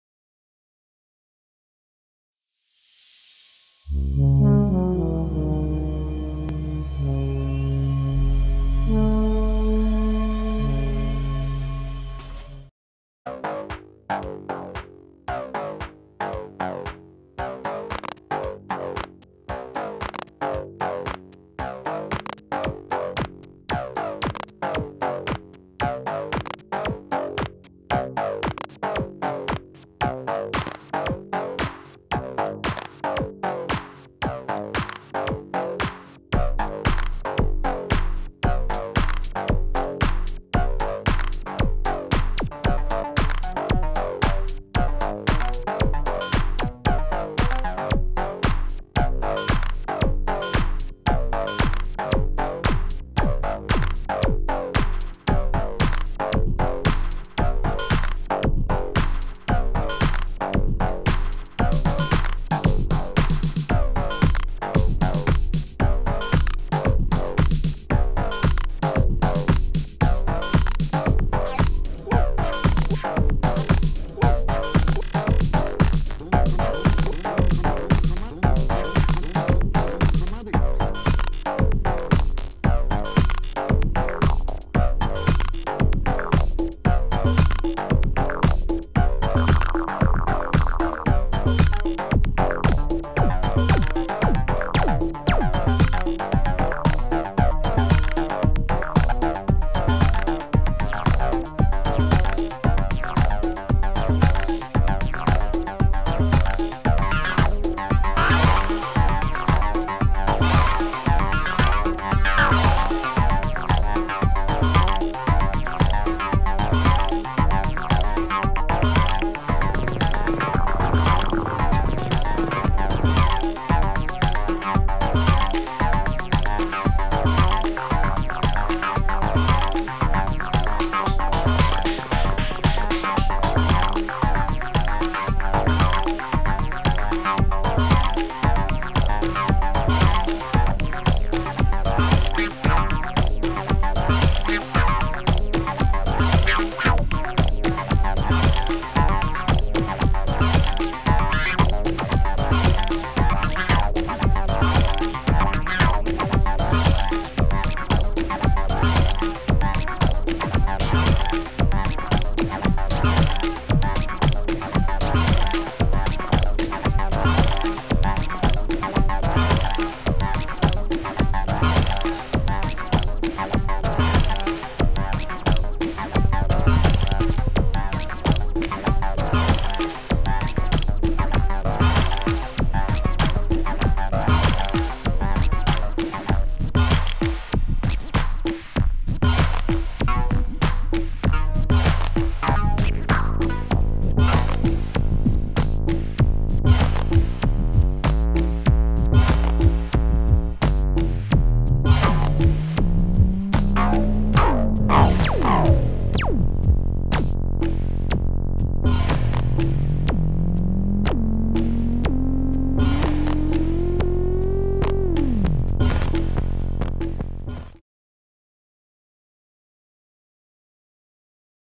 SuperBooth 2016: Musik App Manufaktur Jam
I was on SuperBooth this year for creating video content. More gear news will coming soon.

Not only Modular Synthesizers but also iPad Apps had a nice booth at SuperBooth 2016. Elastic Drums, zMors Modular, Modstep and TriqTraq presented their latest version of their wonderful apps. Here is a little jam with all apps in link mode